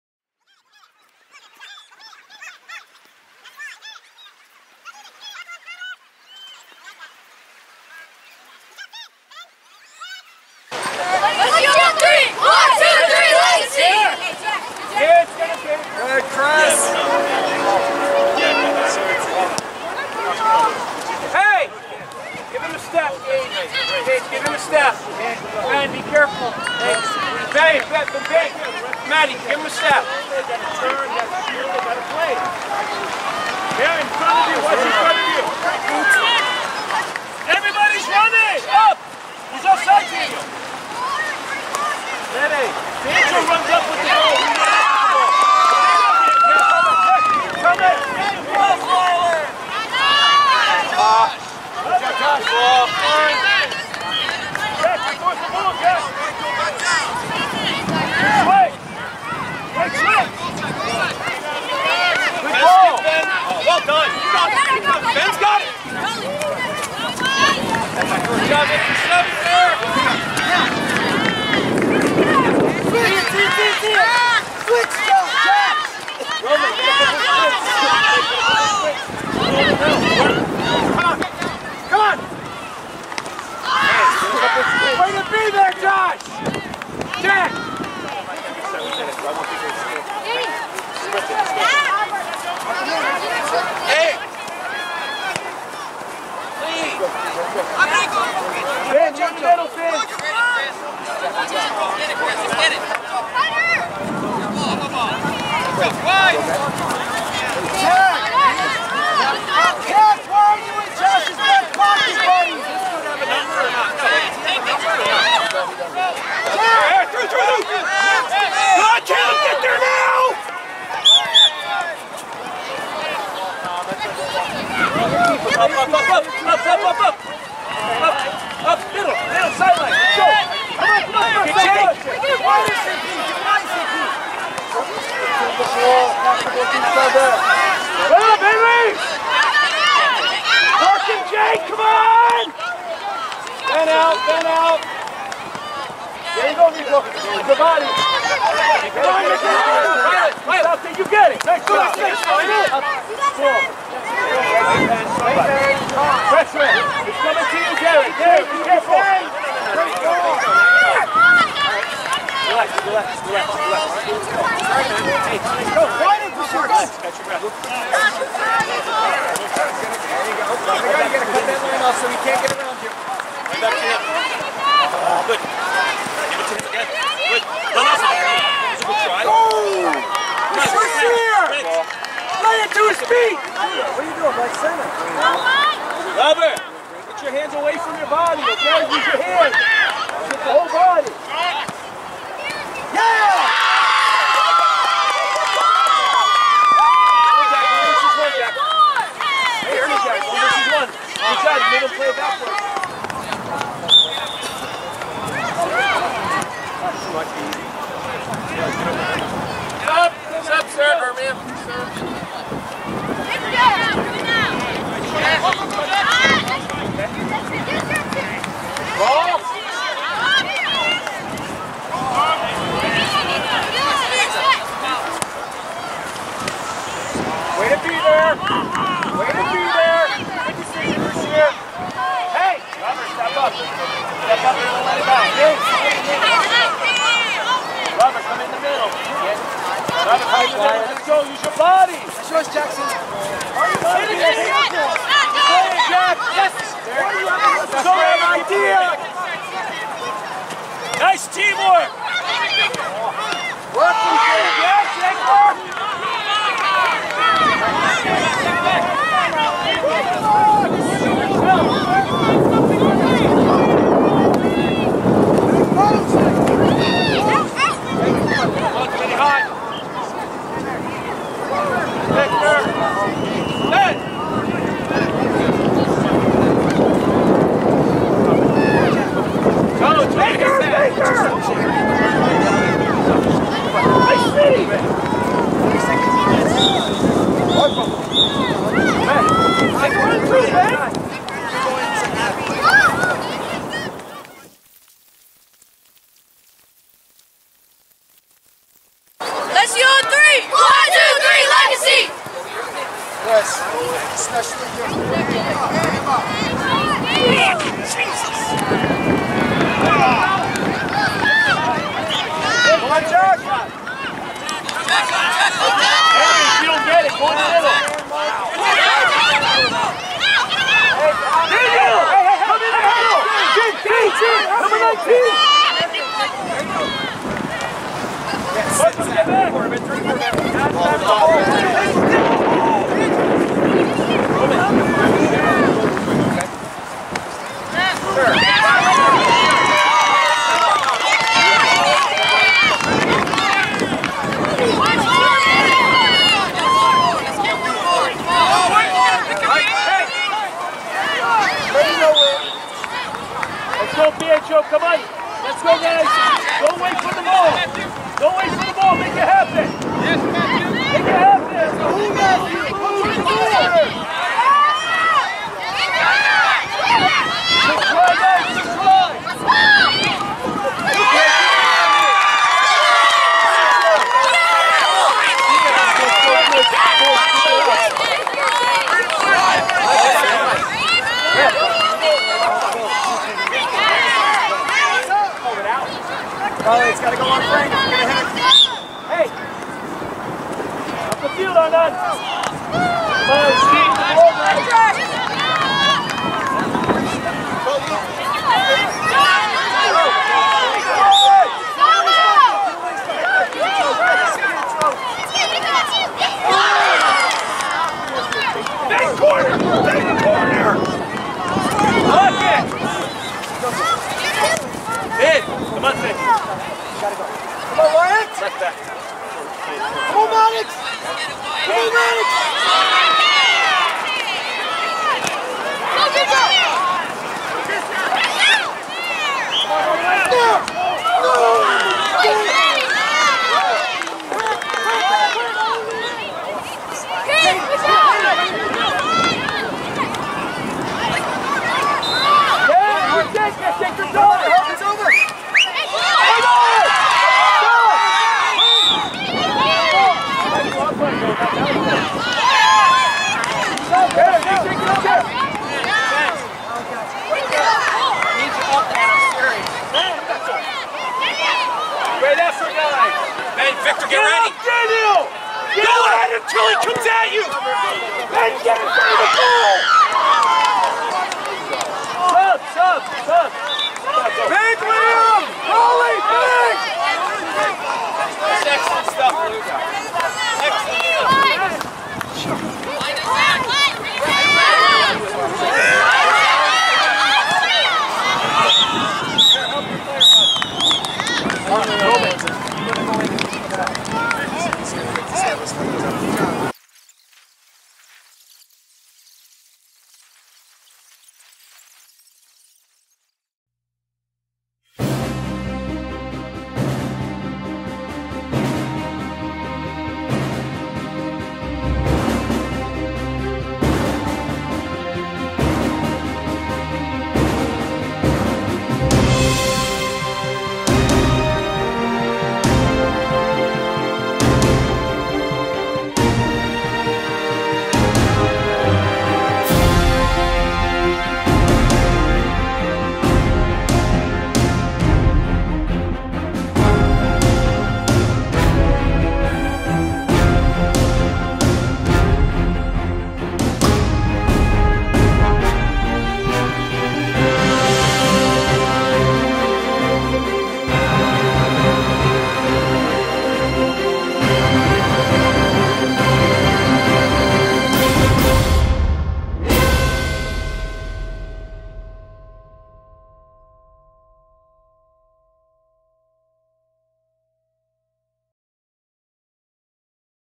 2019-JC: U12 VLSC Elite vs SAC Premier White soccer match (highlights)
Goals at 0:38, 3:08, 5:38, and 6:05 with match statistics at 8:21.